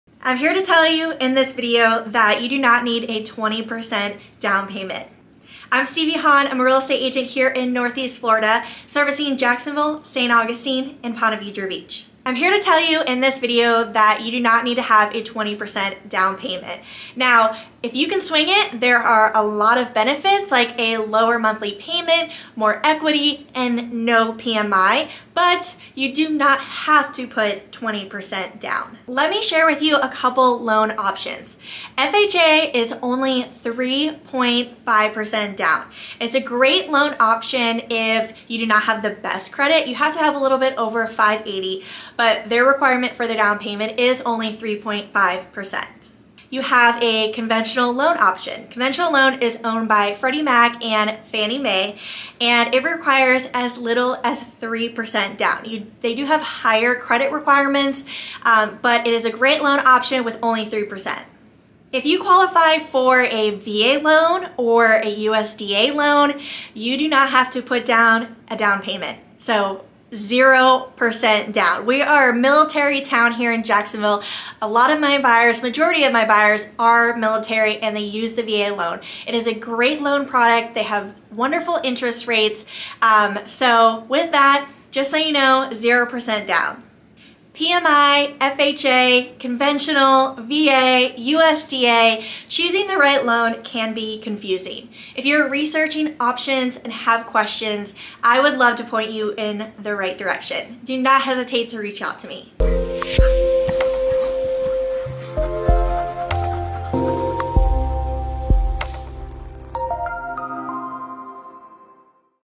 Moving to Jacksonville, FL | You don't have to put down 20%
Serving the Northeast Florida area. This includes Jacksonville, St. Augustine & Ponte Vedra Beach.  This video breaks down the myth of having to put 20% down. I also break down the different loan options.